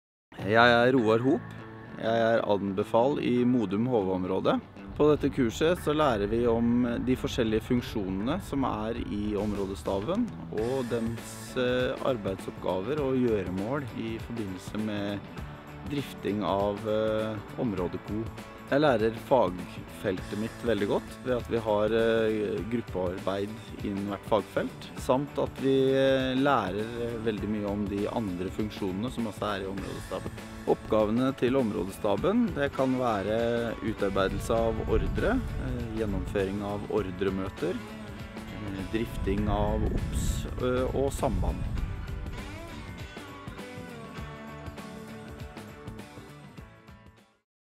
Områdestabskurs i HV
Heimevernets skole- og kompetansesenter skal i 2019 utdanne 3000 befal til område- og innsatsstrukturen.